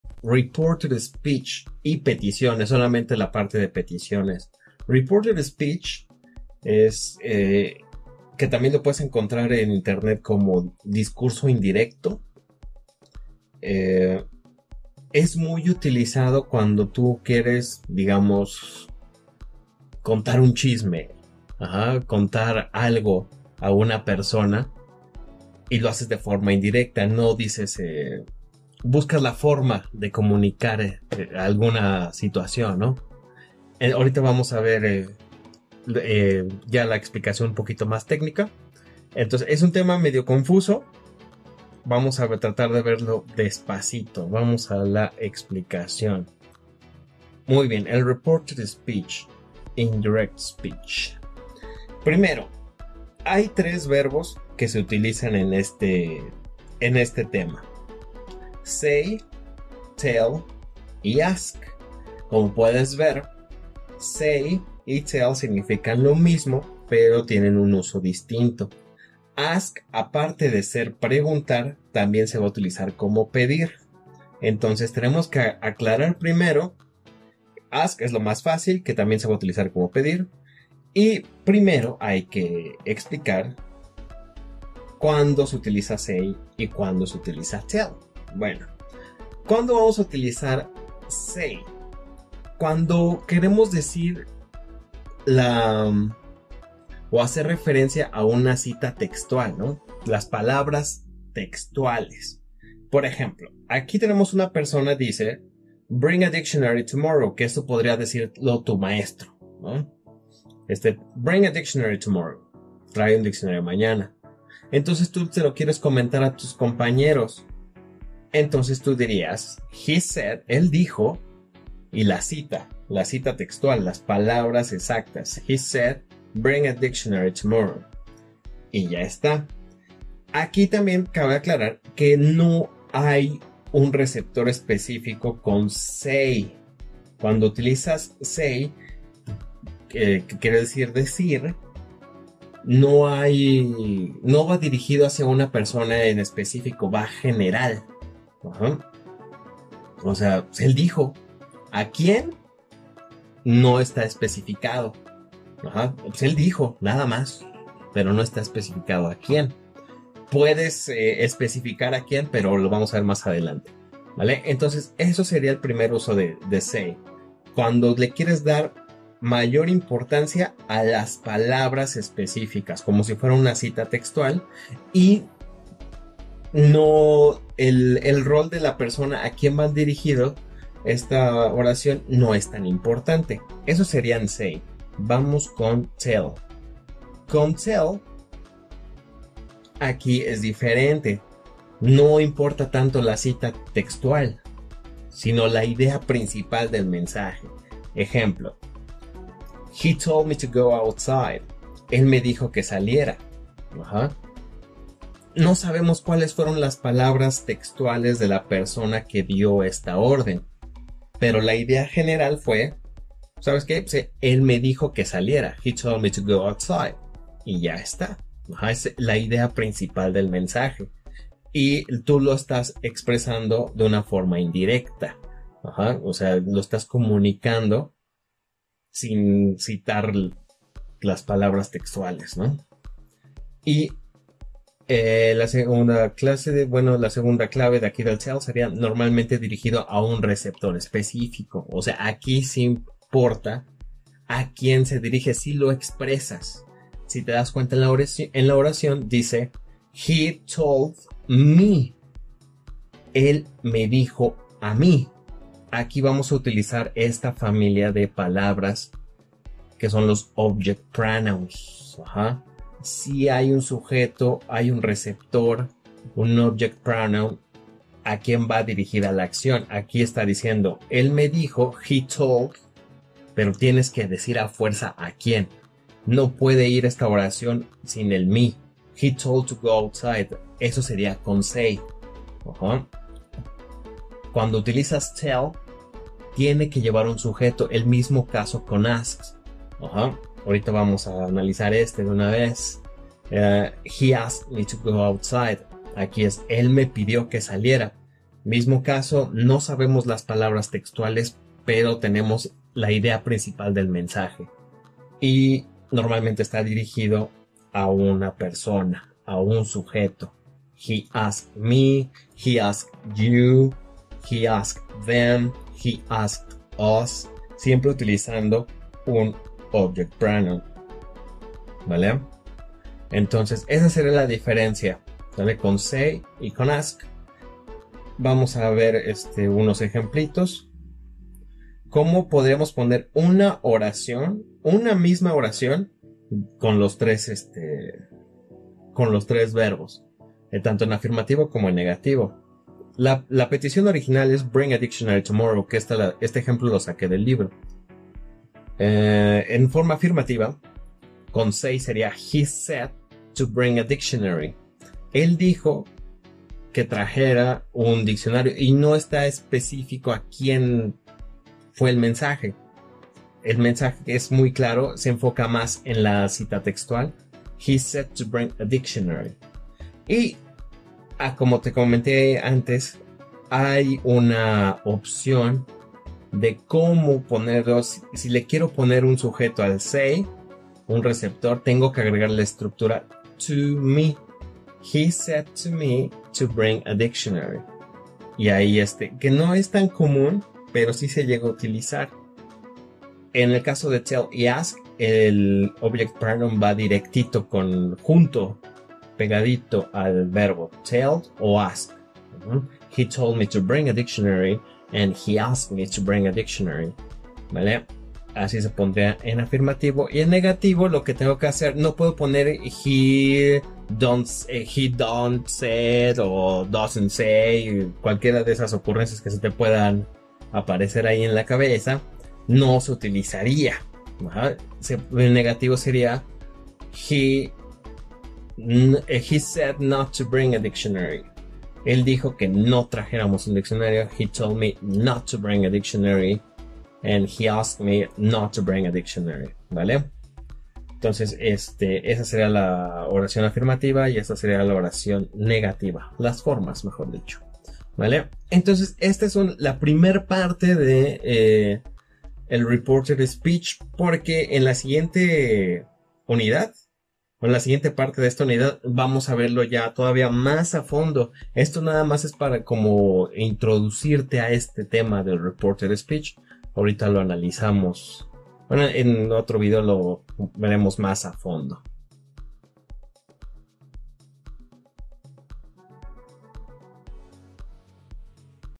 🚨PETICIONES✅SAY vs TELL vs ASK✅REPORTED SPEECH🚨 | EXPLICACIÓN en ESPAÑOL | Nivel B1
NO REPRUEBES tu EXAMEN y MIRA 👈👀este VIDEO✅.
🚨PETICIONES✅SAY vs TELL vs ASK✅REPORTED SPEECH🚨 | EXPLICACIÓN en ESPAÑOL | Nivel B1@TeacherHugoMx

Si quieres que haga un video de un tema especifico o tienes alguna duda, déjalo en los comentarios

Estos videos son extractos de explicaciones mas extensas de mis clases privadas.